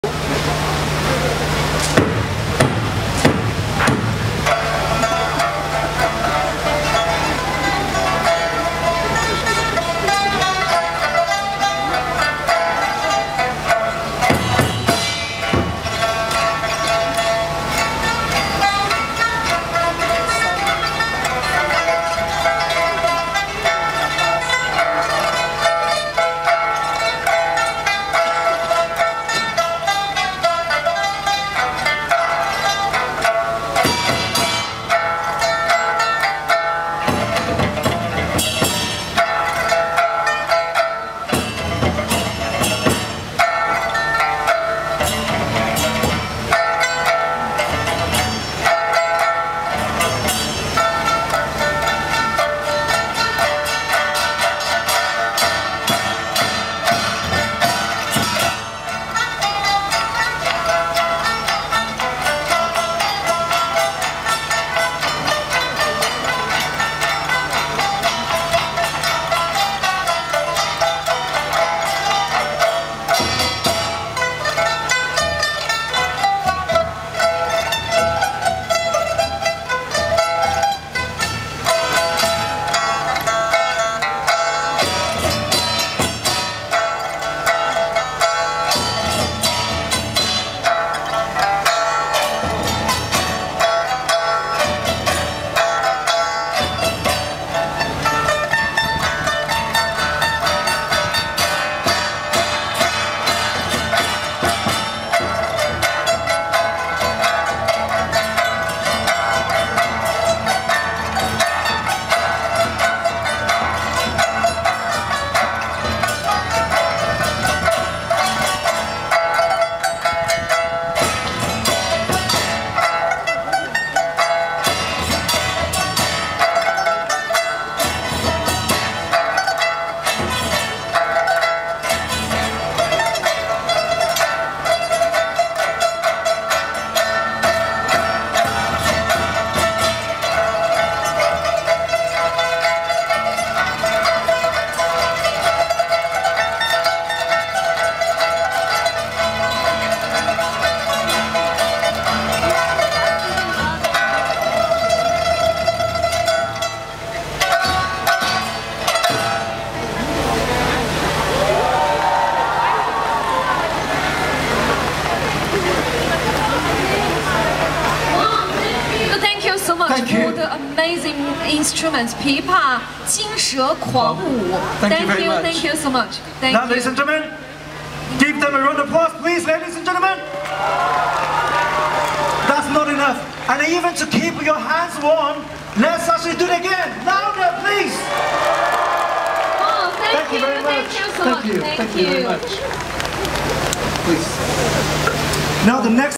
UWTSD Confucius Institute tutor Liu Jing plays the Pipa in Trafalgar Square
Chinese Spring Festival event in London February 2023